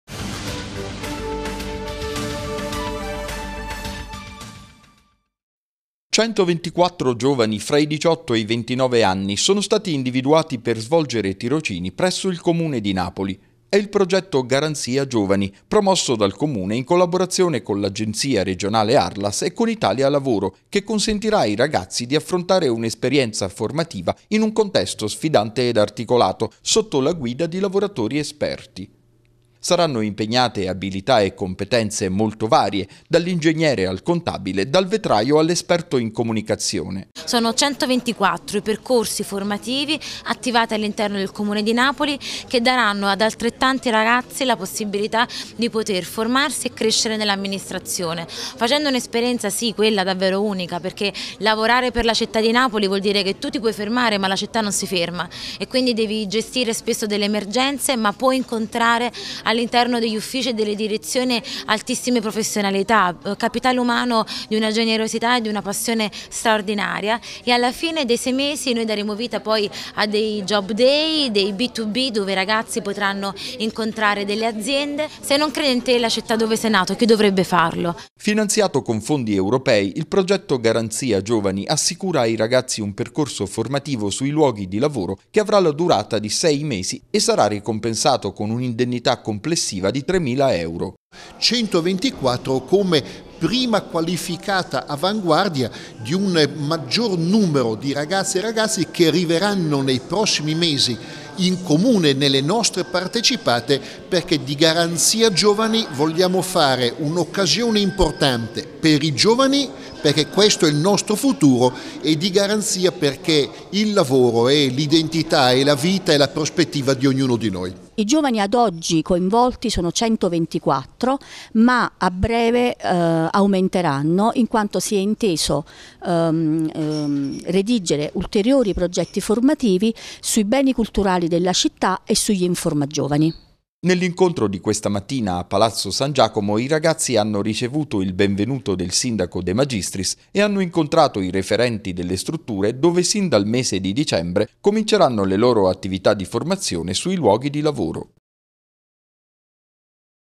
Tirocini al Comune per creare opportunità di lavoro
Centoventiquattro ragazzi di età compresa fra i 18 e i 29 anni parteciperanno ad altrettanti progetti formativi finanziati con fondi europei; i tirocini si svolgeranno al Comune di Napoli e costituiranno un'opportunità per avvicinarsi al mondo del lavoro.